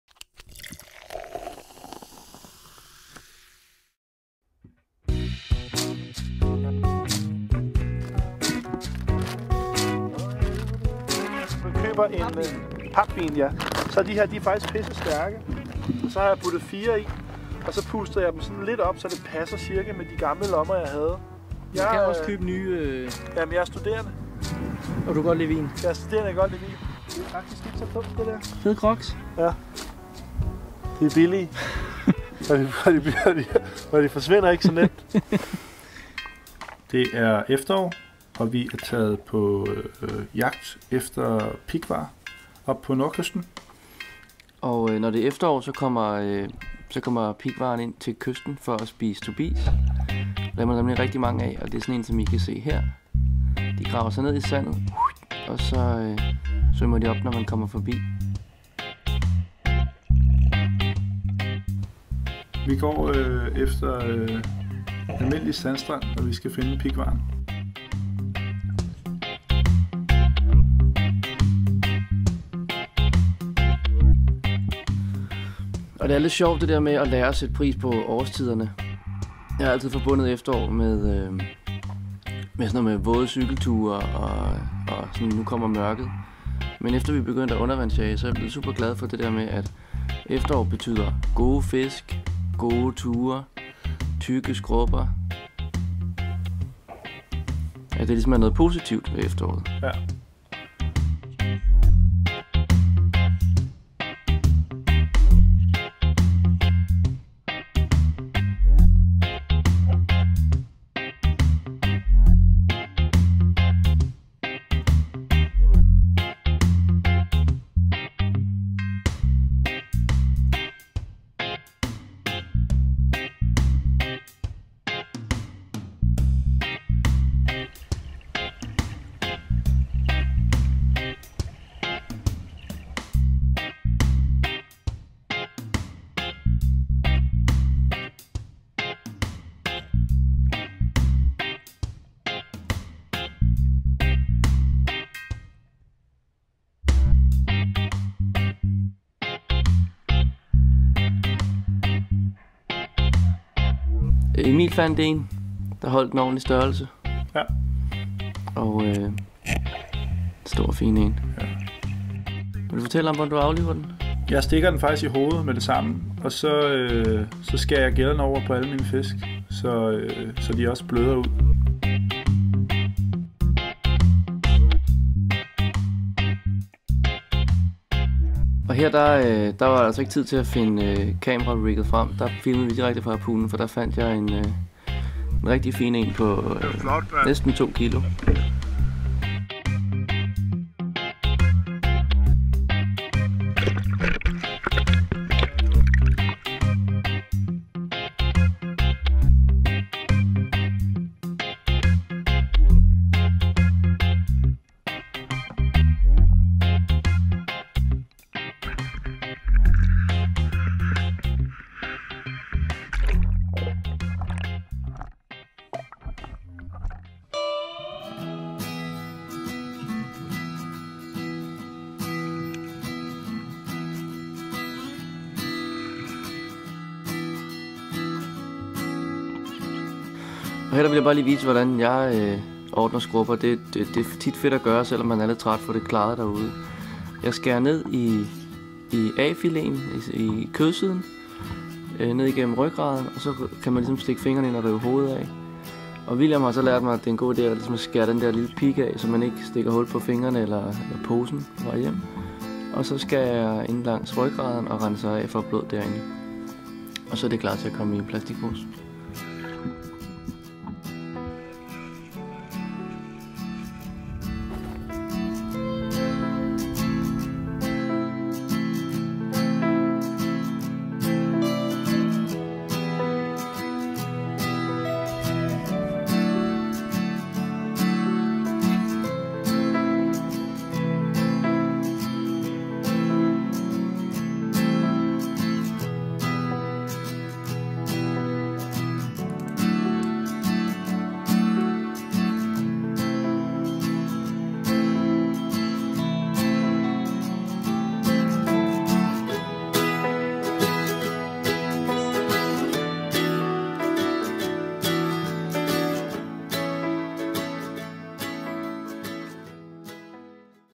Efterårstur efter Pighvarrer - Uvjagt
2/3 af Danskvand Productions, Emil og Jens, er taget på jagt efter pighvarrer på Nordkysten af Sjælland. Det er en tilbagevendende tradition at når efteråret rammer Danmark, skal Danskvand op og søge efter pighvarrer på Nordkysten.
Vinpose-tricket er i øvrigt ét vi har lært af Martin Gottlieb - kongen af "if it ain't broken, don't fix it". :)